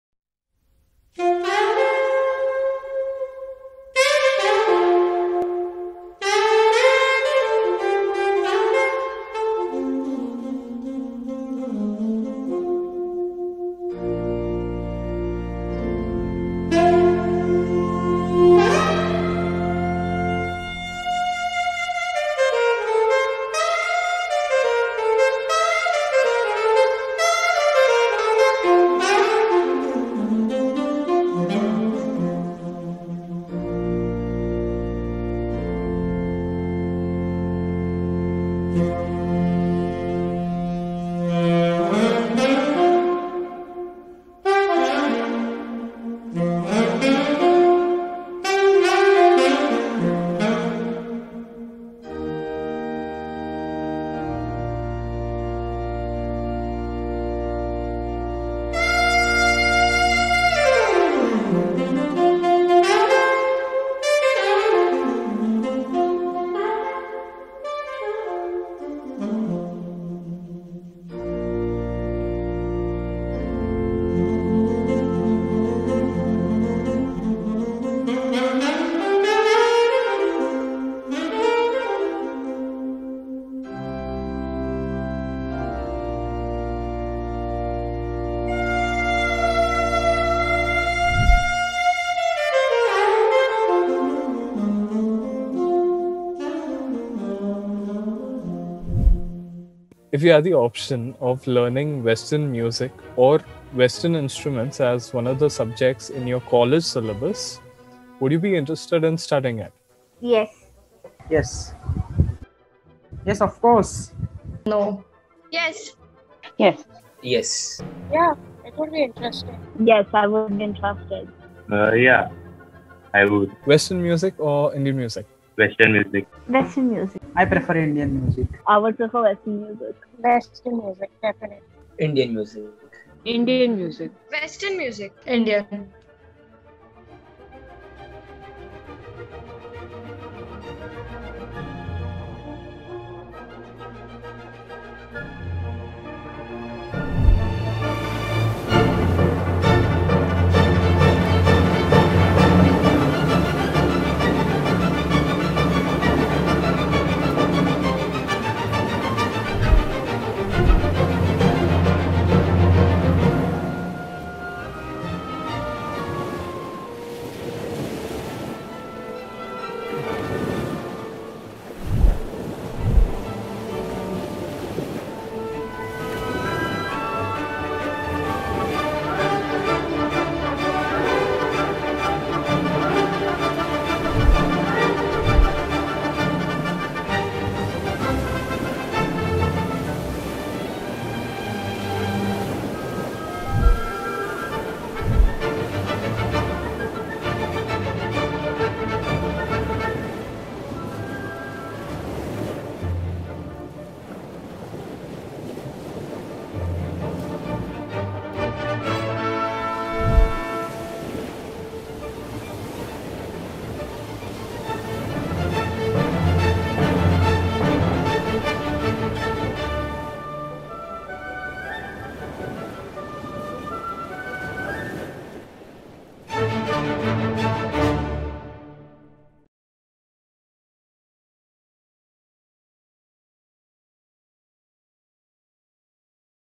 Music of the west- A poetic documentary
Discussing western music education and its future here, in India.